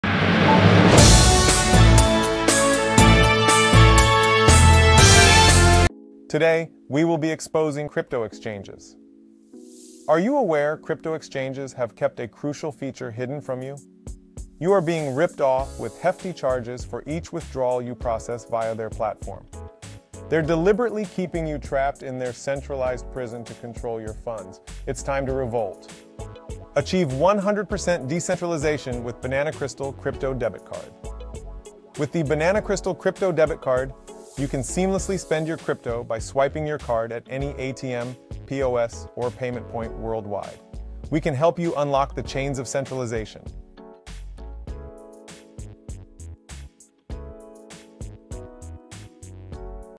Crypto Exchanges are ripping you off!!!
In this video, we reveal the massive rip-off happening in the crypto world by major Crypto Exchanges.

Every time you withdraw funds through their centralized platforms, you get hit with multiple fees.

It's time to REVOLT and BREAK FREE from centralized CHAINS!

Introducing the BananaCrystal Crypto Debit Card – the ultimate solution for seamless crypto asset withdrawals.

to take advantage of this incredible offer.

Join the revolution and say goodbye to excessive fees!